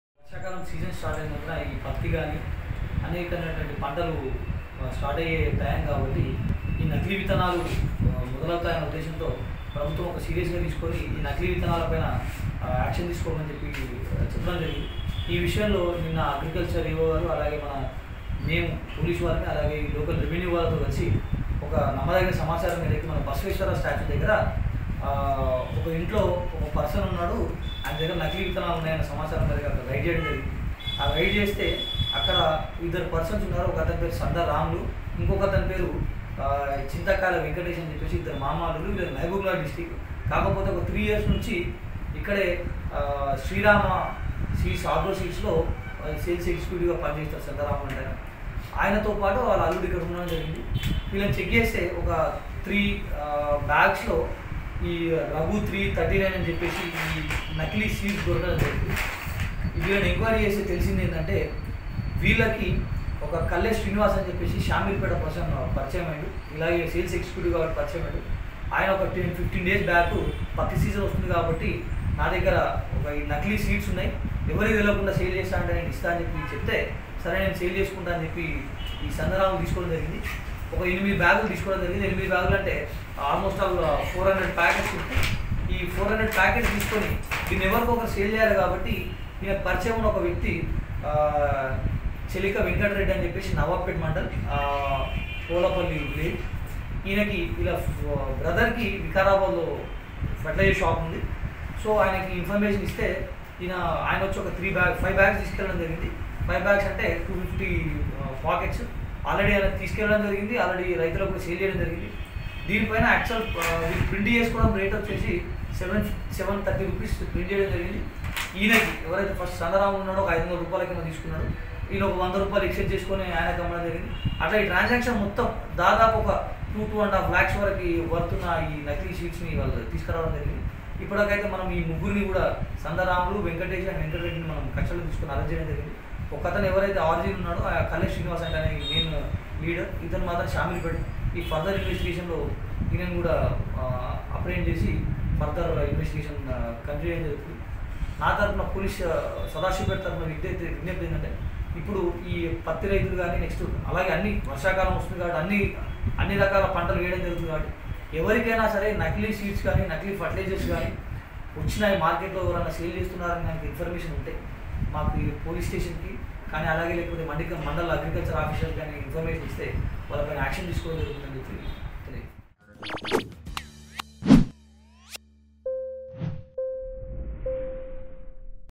సంగారెడ్డి లో నకిలీ విత్తనాల గుట్టు రట్టు | Fake seed sellers arrested in Sangareddy I News18 Telugu
సదాశివపేట నకిలీ పత్తి విత్తనాల అమ్మకాలను గుట్టు రట్టు చేసిన సీఐ శ్రీధర్ రెడ్డి బృందం కేసు నమోదు చేసి ముగ్గురిని రిమాండ్ కు తరలించిన పోలీసులు.